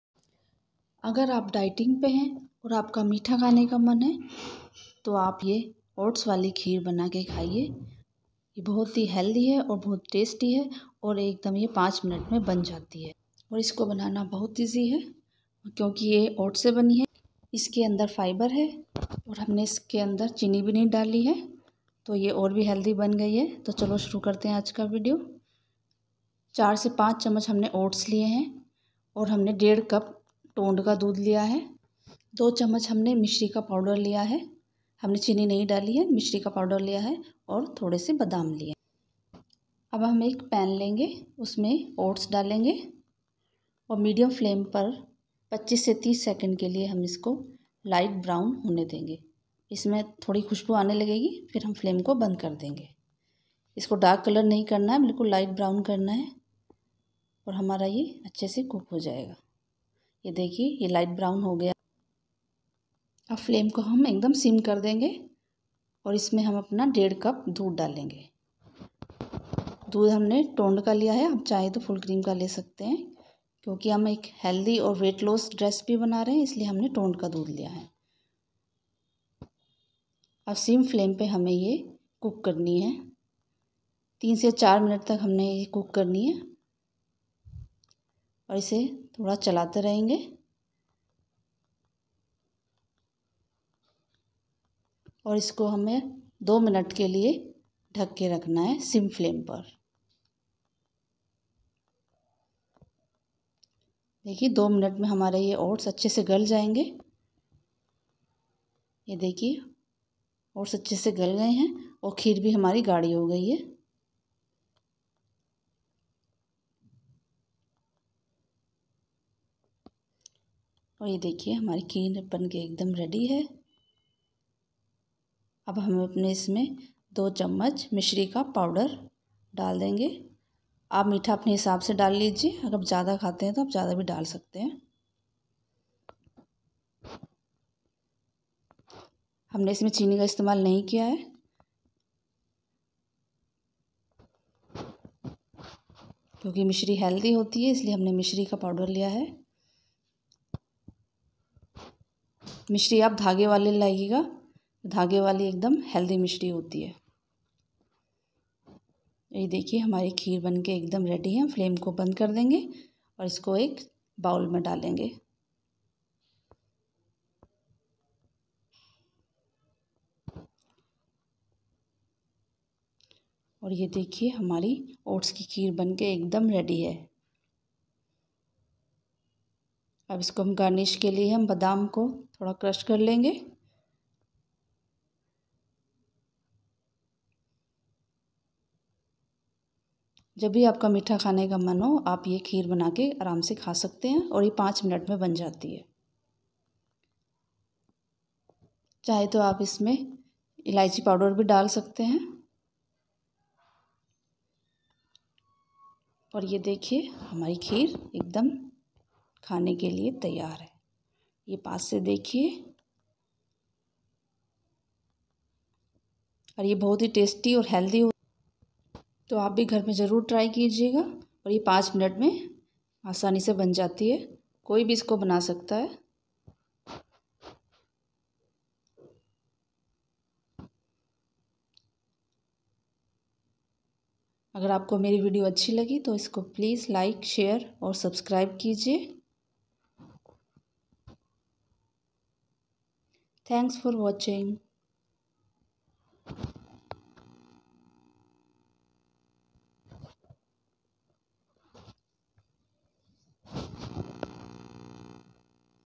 Oats की खीर बिना चीनी के बनायें केवल 5 मिनट में।Weight loss recipe @pure food station
This channel is based on home made food which is tasty & good for health.

Ingredients List :-
1. Oats - 4  spoon
2. Milk - 1 1/2 cup
3. Mishri powder - 2 spoon
4. Almonds - 4-5 only

Oats की खीर बिना चीनी के बनायें केवल 5 मिनट में। Weight loss recipe@purefoodstation